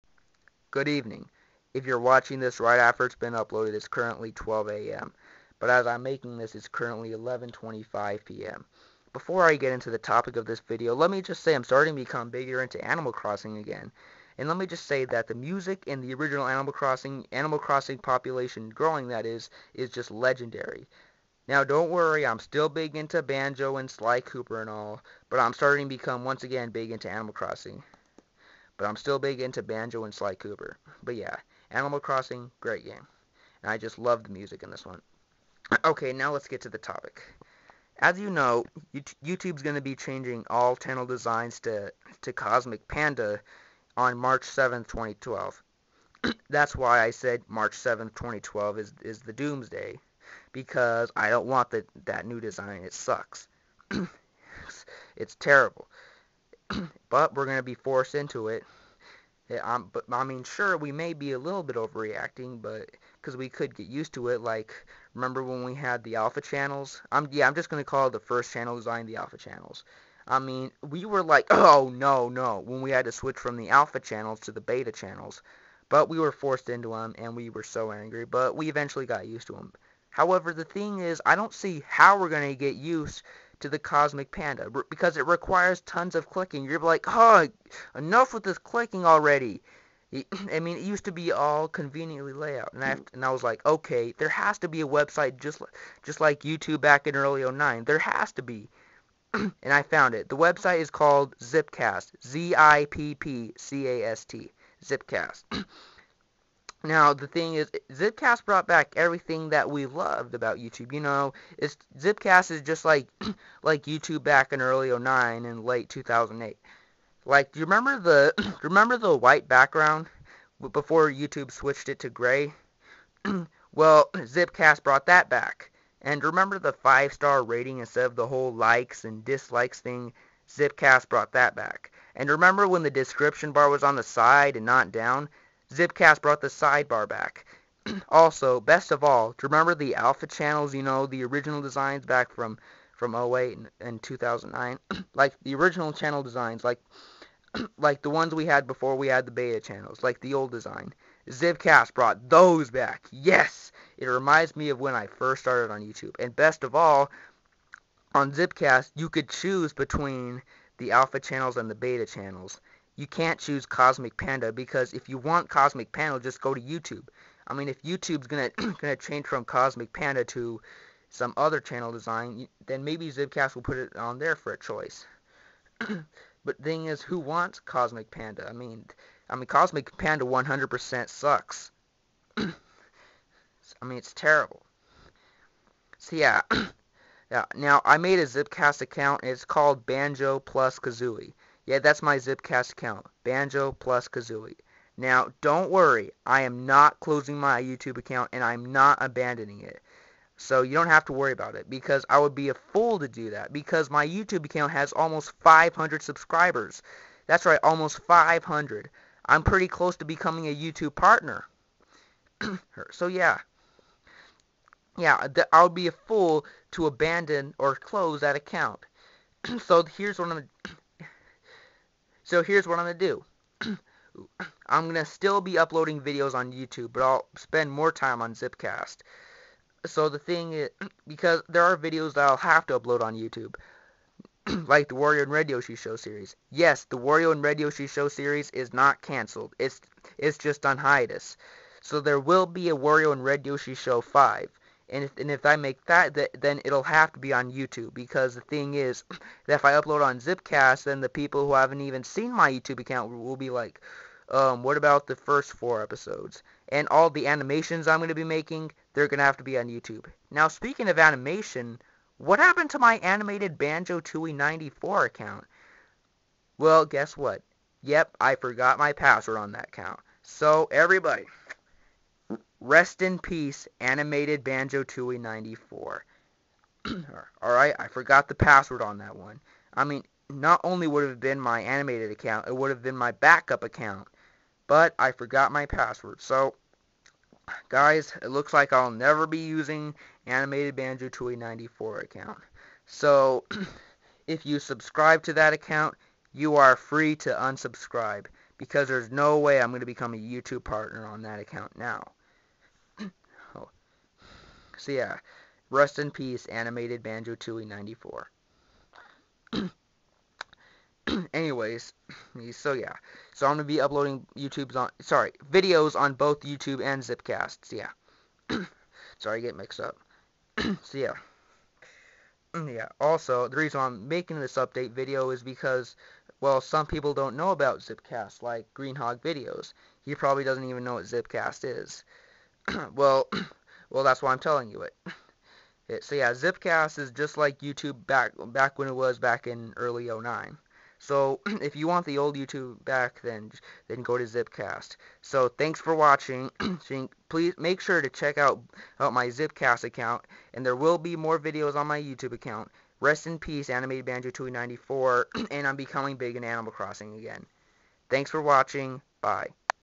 Update - January 29, 2012
This is an update video about YouTube and ZippCast.

Also, I'm becomming big into Animal Crossing again!